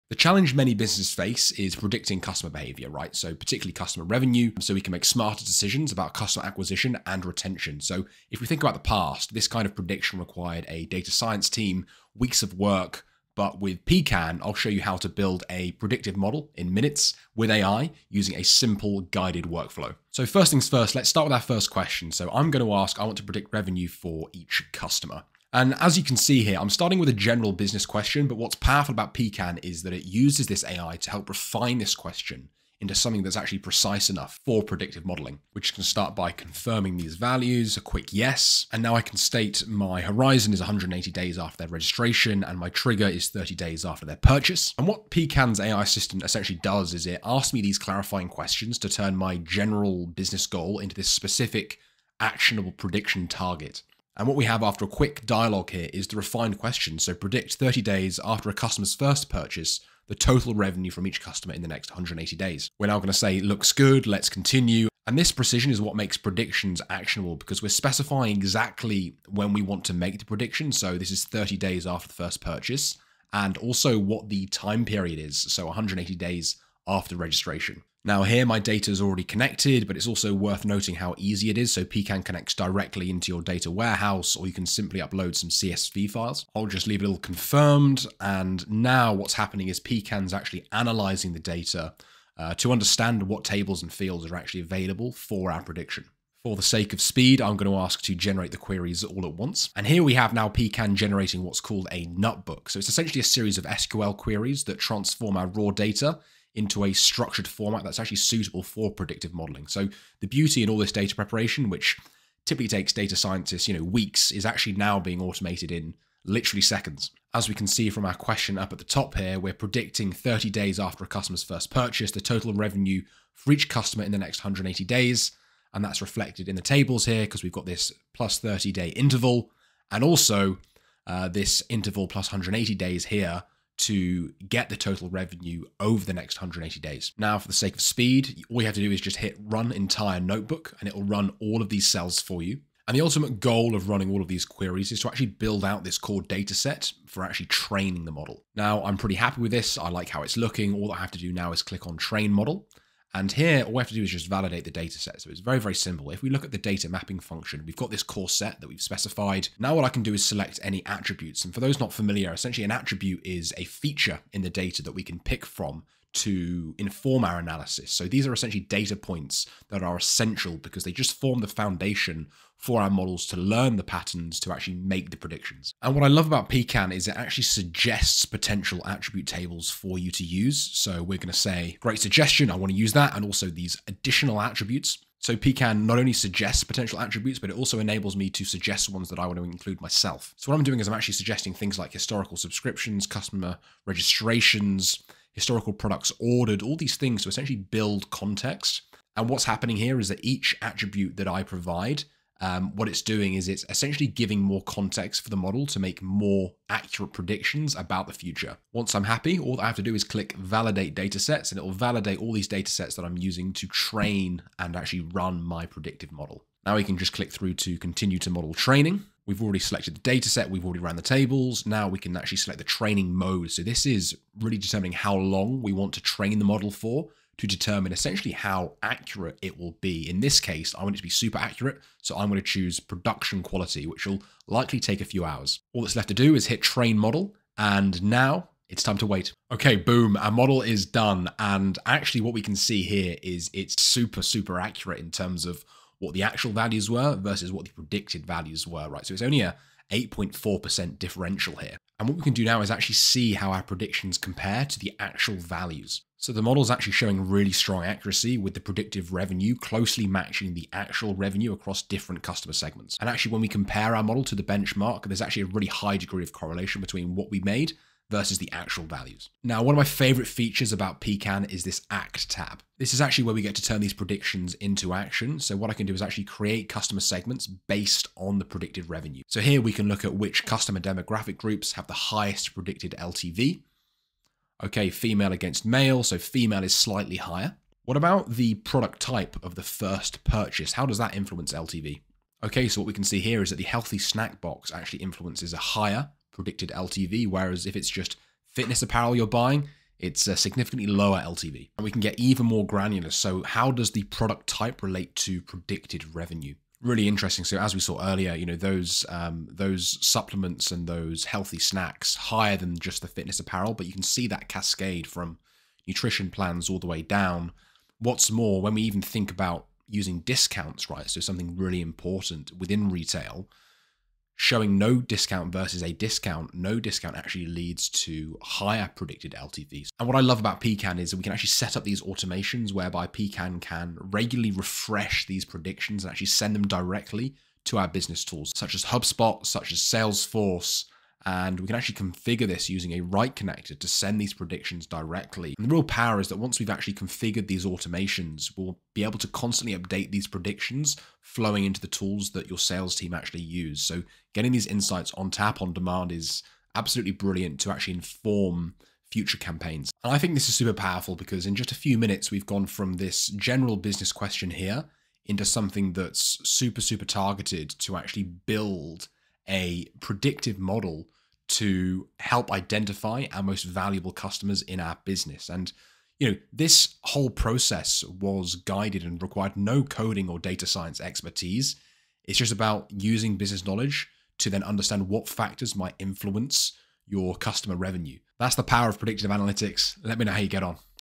Alex Banks | Predict Customer Behavior with AI in 10 Minutes | No-Code Pecan AI Tutorial
Stop guessing - start predicting. In this quick guide, Alex Banks will show you how anyone can forecast churn, spending patterns, and more without writing a single line of code.

What you’ll learn:
 1. Turn a business question into a predictive query
 2. Connect CSVs or data warehouses in a click
 3. Auto-generate a full SQL + feature-engineering notebook
 4. Train & validate models in 10-30 minutes
 5. Push predictions straight into HubSpot, Salesforce, Marketo

Why it matters: AI is the great equalizer, letting non-technical teams unlock data-powered decisions fast.

Ready to try it?